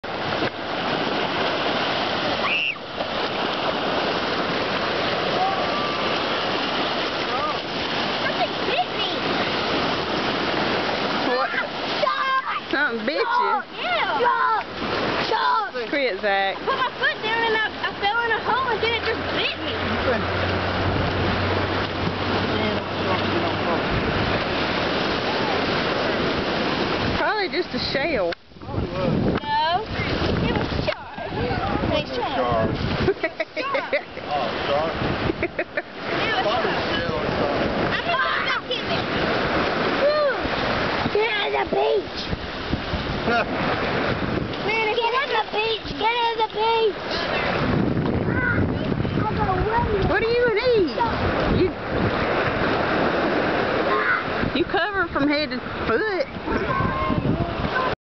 something bit me
10 year ol dgets bit by a crab i'm thinking. the same crab got me i also. or maybe it was his cousin??